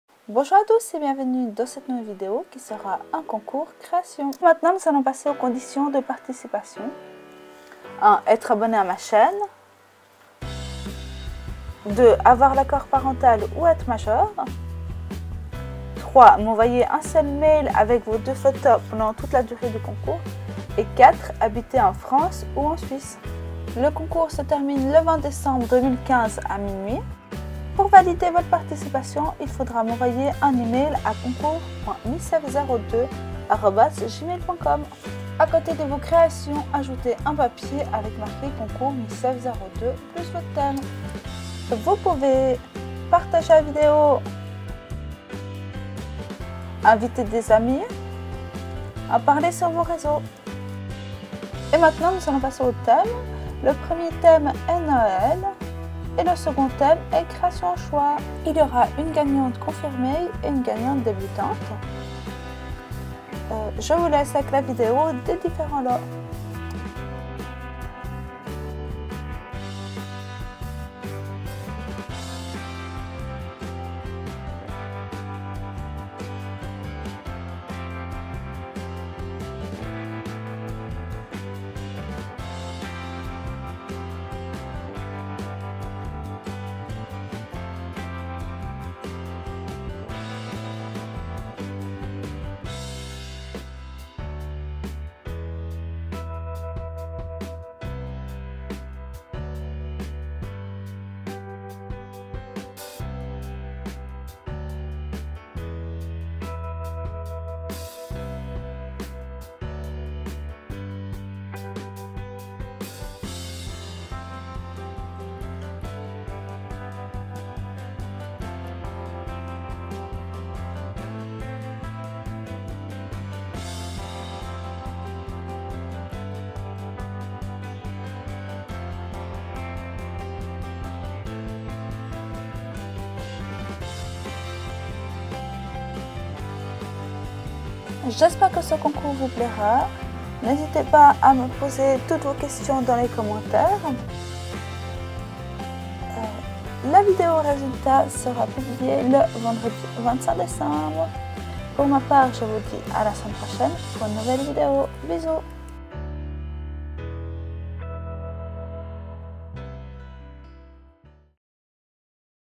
Concours#2 Créations - [FERMER] | MissSév 02
{ Pour plus d'informations déroule }

Participer a mon 1er concours Création sur le thème de Noël. J'attends vos participation. :-)

Conditions :
- Ne pas mettre je participe dans le commentaire - juste les questions
- Être abonné(e)
- Avoir l'accord parental ou être majeur
- M'envoyer un seul mail durant toute la période du concours
- Habiter en France Métropolitaine ou en Suisse

M'envoyer à :
Ajouter confirmé(e) ou débutant(e)

Je film avec un Nikon one J3
Je monte mes vidéos avec Imovie 10